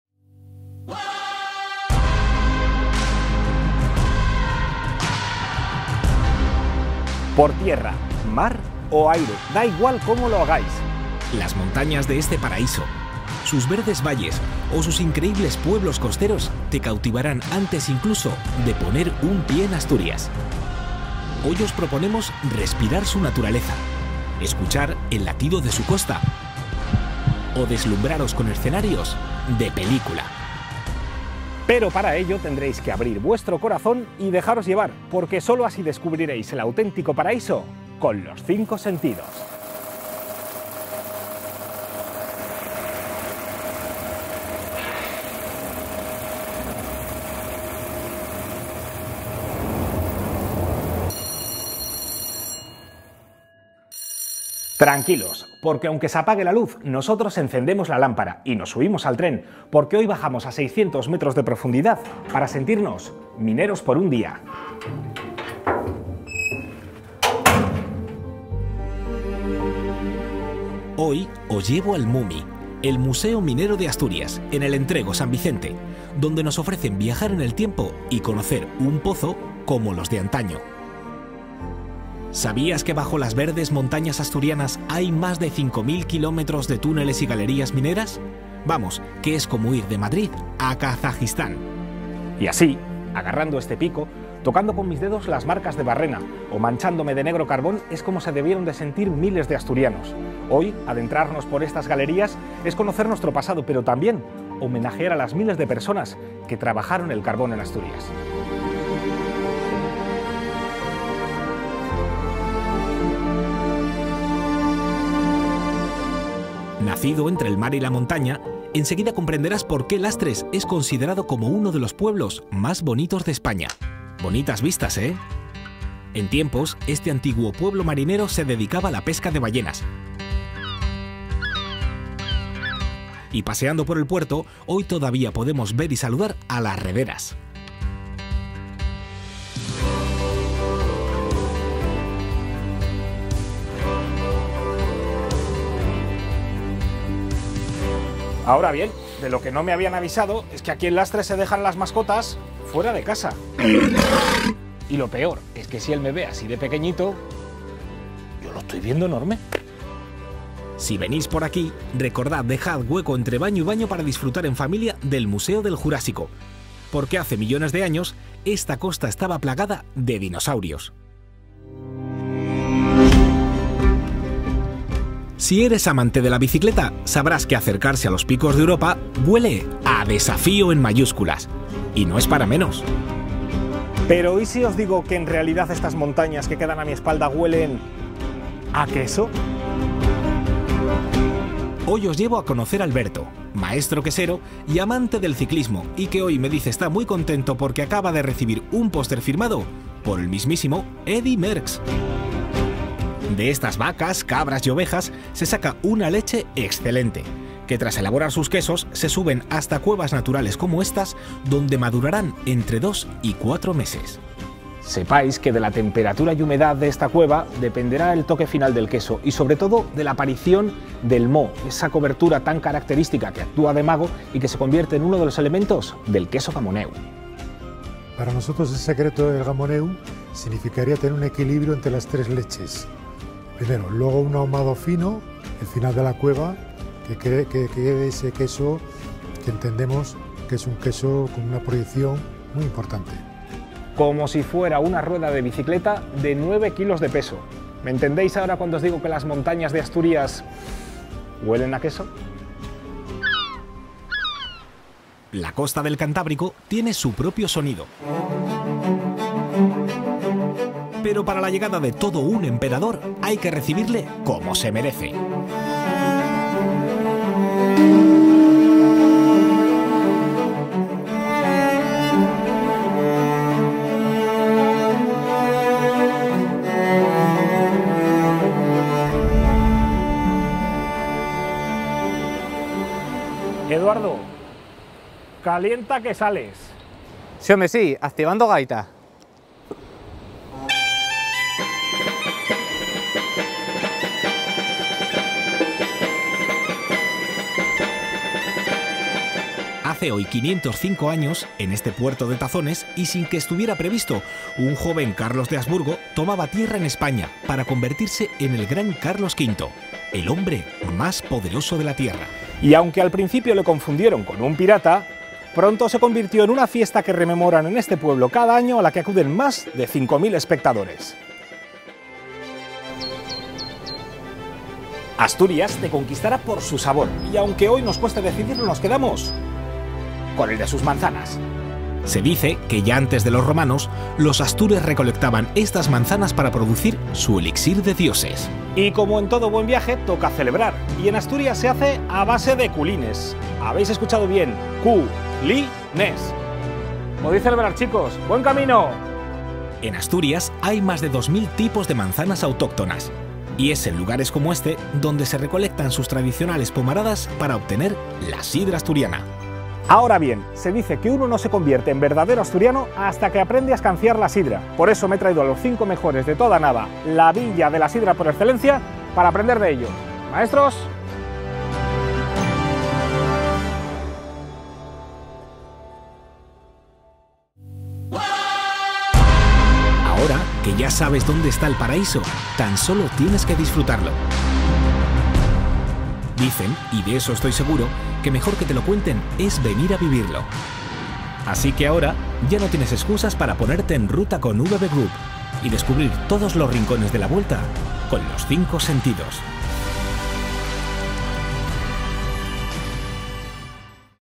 La Vuelta de los Sentidos | Asturias
🇪🇸 - Por tierra, mar o aire, da igual cómo lo hagáis. Las montañas de este paraíso, sus verdes valles, o sus increíbles pueblos costeros, te cautivarán incluso antes de poner un pie en Asturias. Hoy os proponemos, respirar su naturaleza.  Escuchar el latido de su costa. O deslumbraros con escenarios de película. Pero para ello tendréis que abrir vuestro corazón y dejaros llevar, porque solo así descubrir el auténtico paraíso, con los 5 sentidos.

🇬🇧 - By land, sea or air, it doesn’t matter how you do it. This paradise, with its mountains, its green valleys and its incredible coastal towns, will captivate you even before you set foot in Asturias. Today, we propose that you breathe in all of its nature. Listen to its coastal heartbeat. Or be dazzled by its amazing sceneries. But, in order to do this, you’ll have to open up your hearts and let yourselves be carried away, because only then will you discover a true paradise, with your 5 senses.